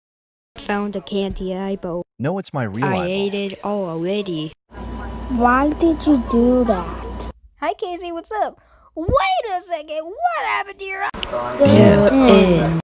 Voicing over your fan animations with me and/or my characters in them part 21!
Keep making those fan animations to keep this series going!

Original animations l belongs to @FunTimeWithZoobie!